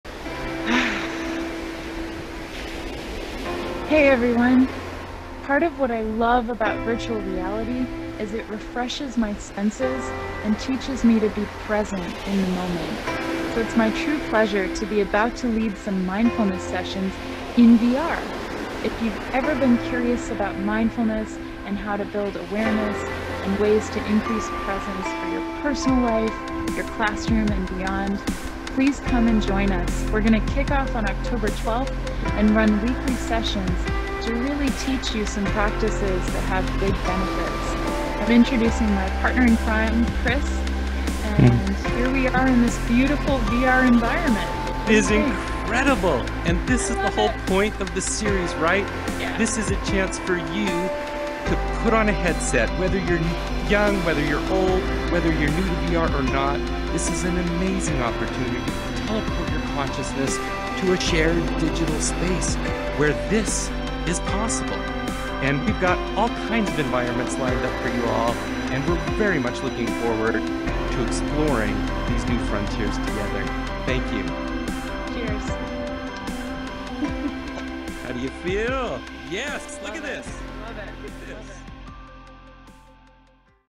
Series Announcement: Mindfulness in Virtual Reality
"Mindfulness in VR" Experience Series
Starting October 12, weekly live sessions

Join us to learn and practice the principles of mindfulness from Caitlin Krause, acclaimed author and educator. Caitlin's passion for empowering connection and using virtual reality to amplify presence make this course a breakthrough event series.

If you're new to mindfulness (or new to VR!), this series will be a crash course in powerful ways it can impact your life. If you're a seasoned enthusiast, this is sure to deepen your practice. Come to boost your creative awareness, social-emotional skills, presence and well-being. Join us to explore some of the ways VR is unlike any other environment. Arrive, "be" in community, and celebrate the chance to stretch, enjoy, and appreciate a medium that seems perfectly made for mindfulness!

Each week is a live interactive session, led by Caitlin and hosted by VR trailblazer Chris Madsen. You'll be treated to beautiful environmental landscapes, plus enjoy active engagement in mindfulness practices you can use in personal and professional life.

Space is limited; access signups and more details